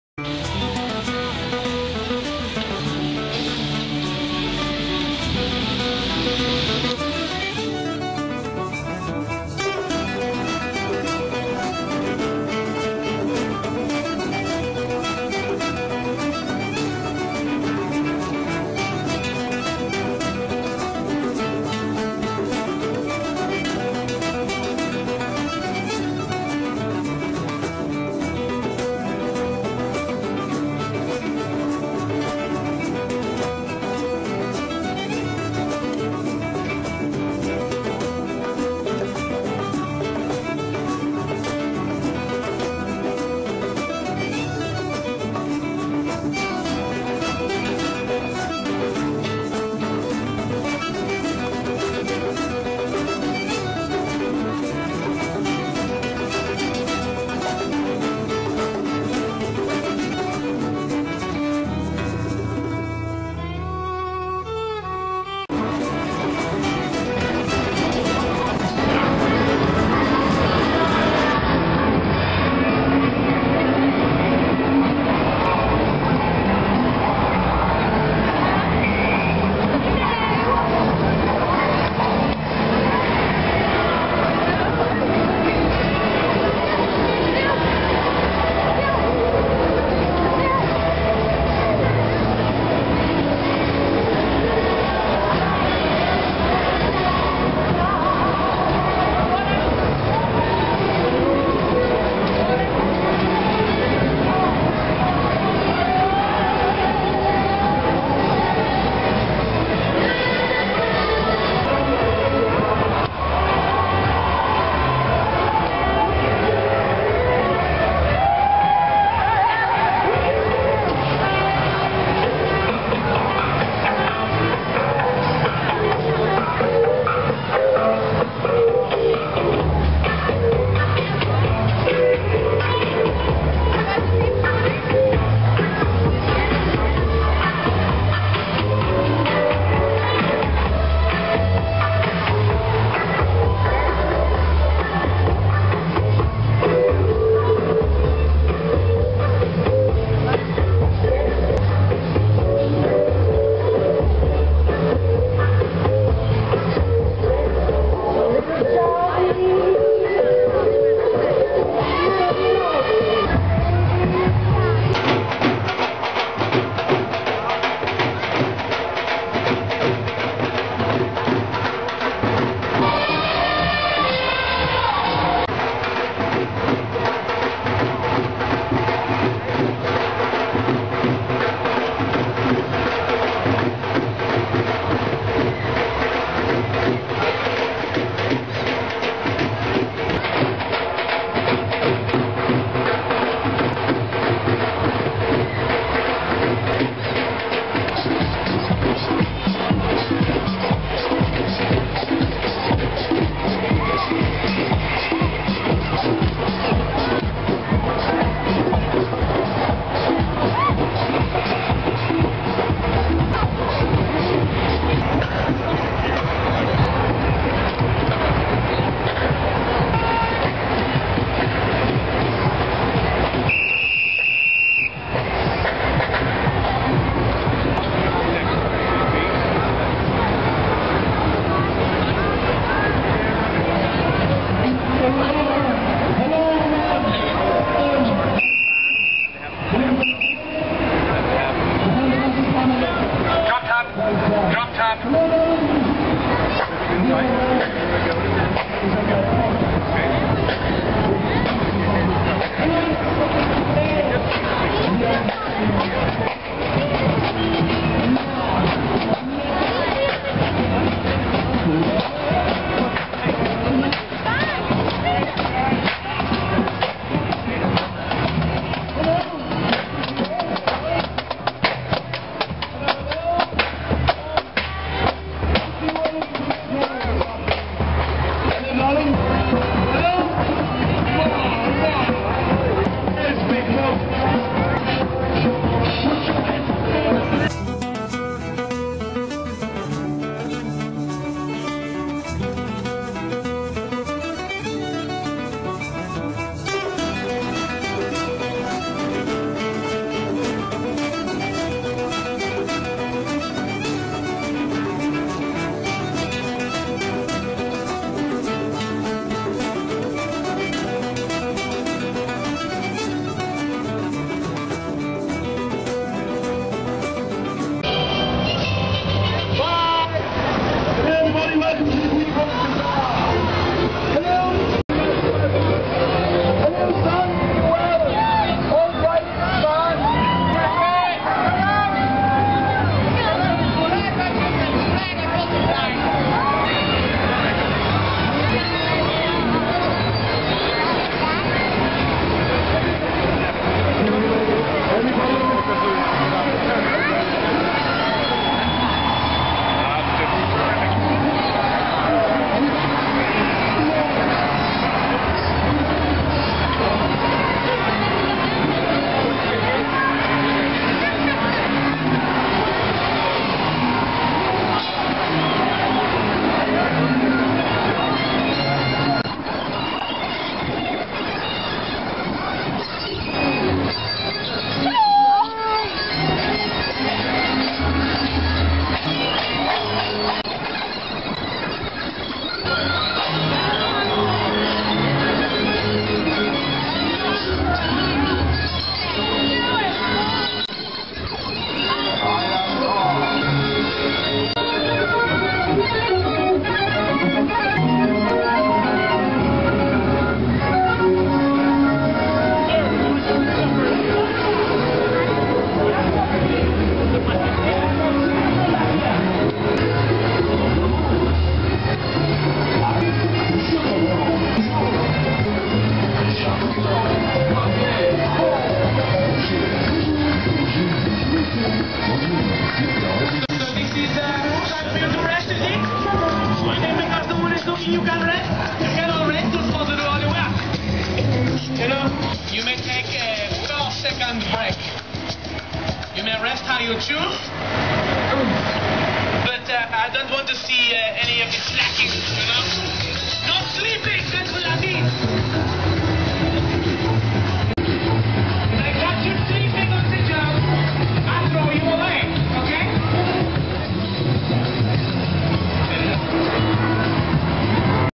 St Patricks Day Anniversary Parade, Dublin, Grafton Street, Ireland Public St Paddys Day
Part 3 of the Special Video Coverage of 15th anniversary of St.Patrick's Day at Grafton Street, Dublin, Ireland...